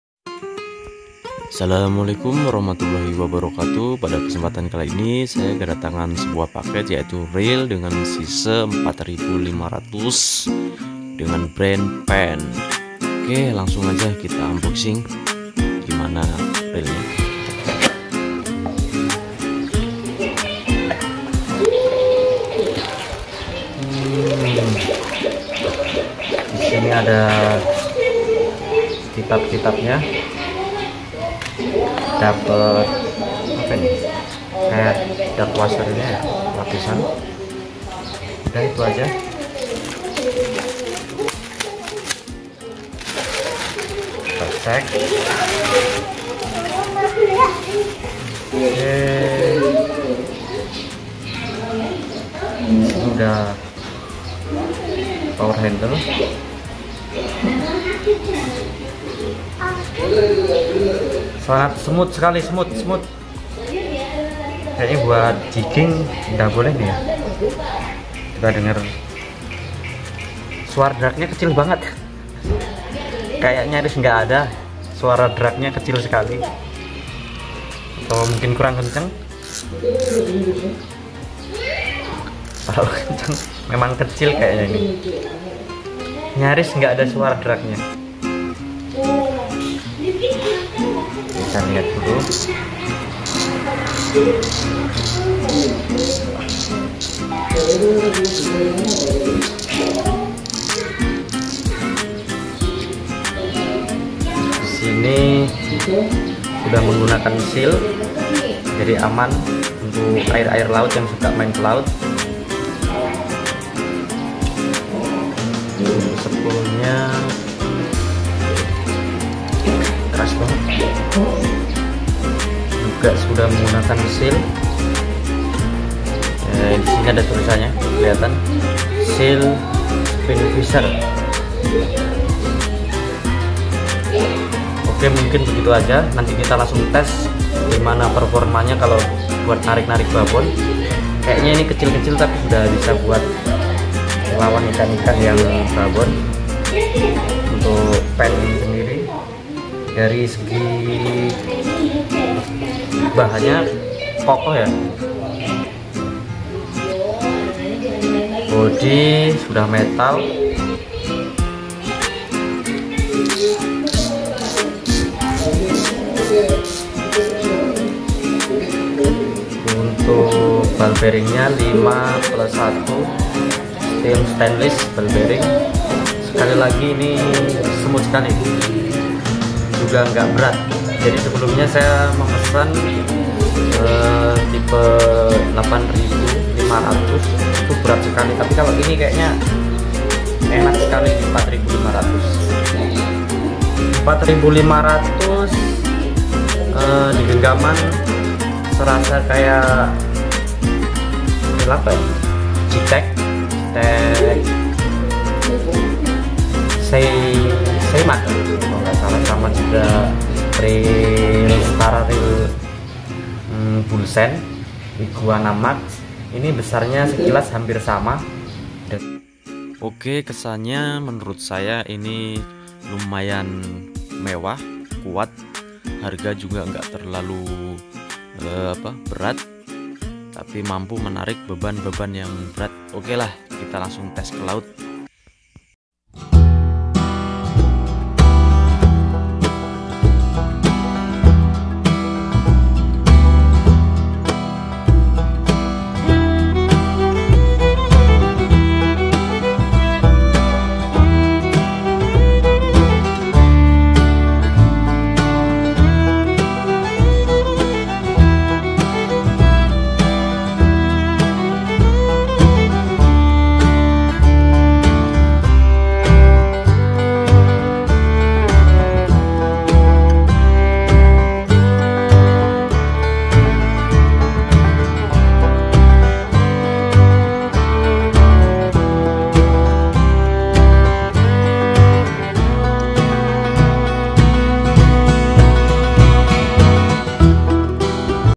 REEL PENN LEGEND USA KUAT KOKOH DAN SMOOT
ini reel penn spinfisher v  yang baru saya beli karna penasaran,dengan produk penn tersebut,oke kali ini saya unboxing reel penn spinfhiser dgn zize 4500